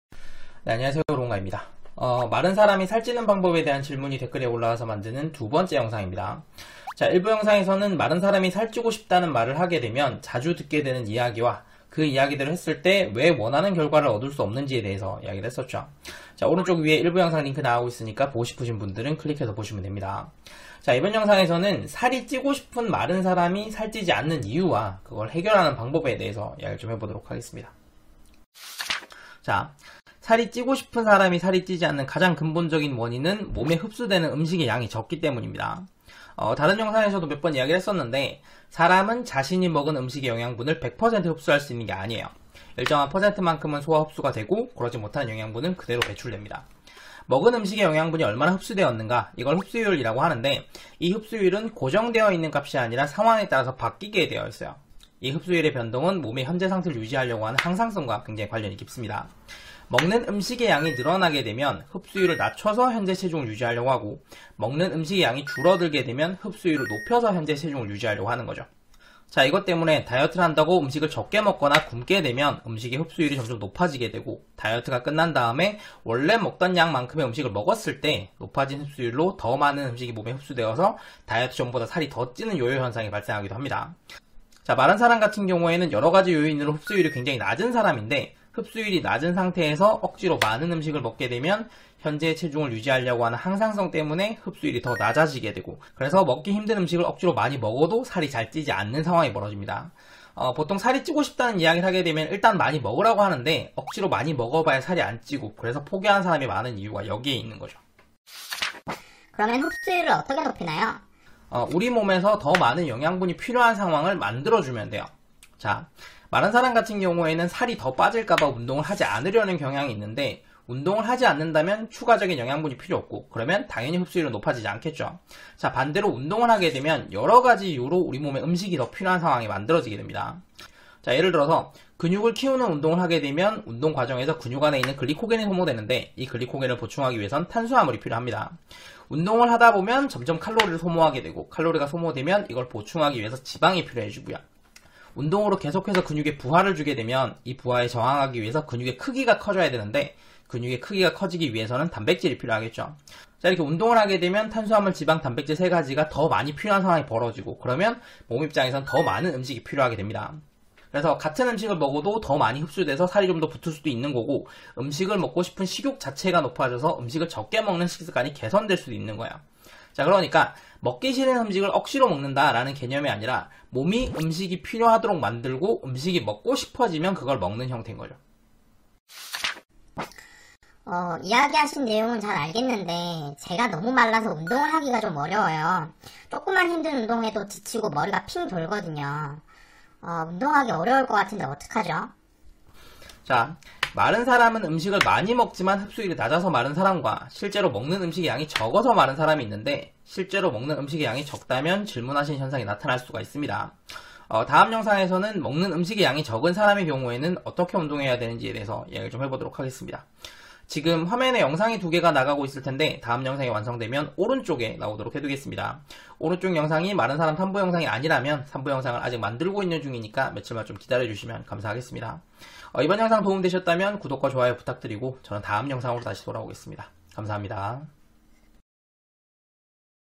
살이 안찌는 이유와 살찌는 법 - 마른사람 2부
모든 영상은 유튜브 기능으로 자막이 제공되고 있습니다
자막이 필요하시면 유튜브 자막을 활성화 해주세요

살이 찌고 싶은데 음식을 많이 먹어도 살이 안찌거나
오히려 속이 불편해서 고생하는 사람이 많습니다
마른사람이 살지지 않는 근본적인 이유와 해결 방법을 정리해봤습니다